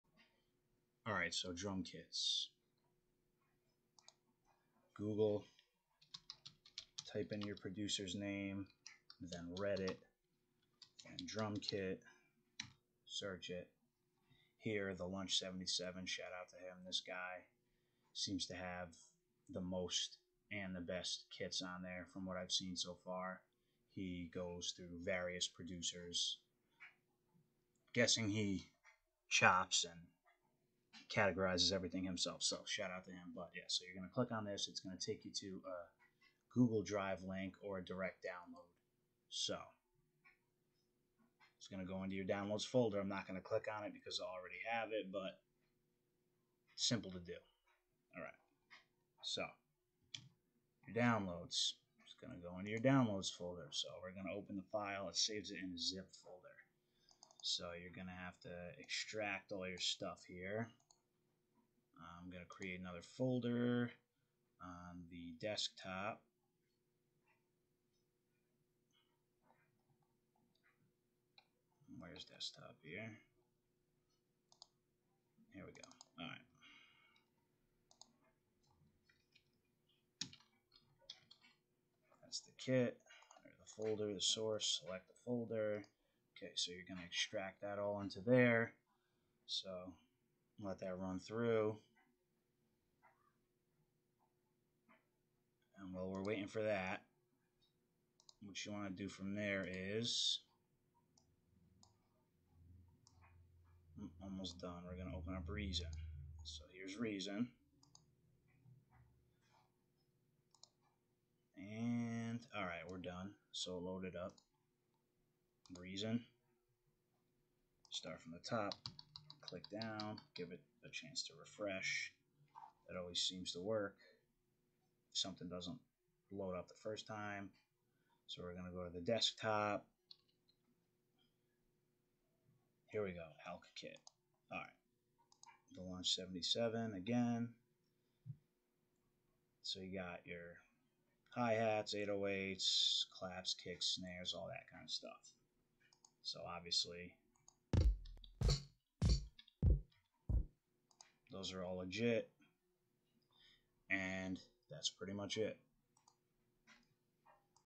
Reason 12 Drum Kit Downloads
Easily find drum kits to download for free to use in Reason Kong or Sequencer